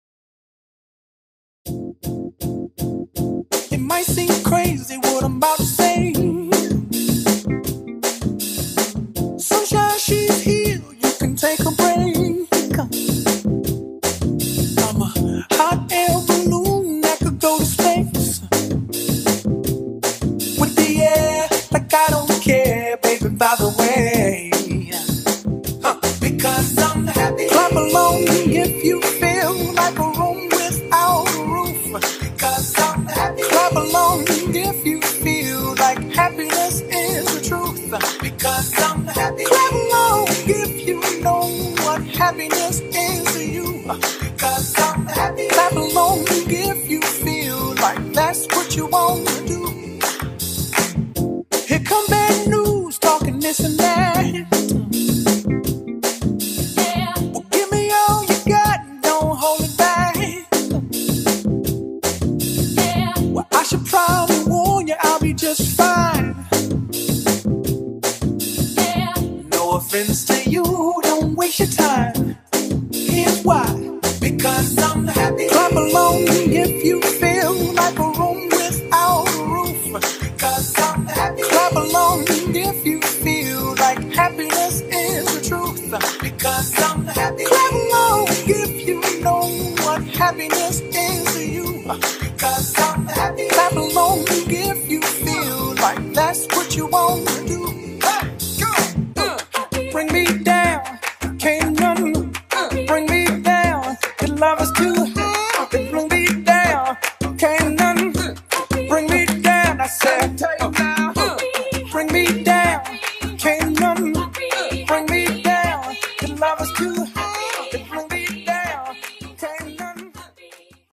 1-14-18 Atlanta 103 West Photo Booth - Annual Buckhead Life Party - Robot Booth
Robot Booth, Atlanta's best photo booth, was at 103 West for the Annual Buckhead Life General Manager & Executive Chef Party!

Look for the gallery link below to see all the pictures taken and get downloads of your pictures for free!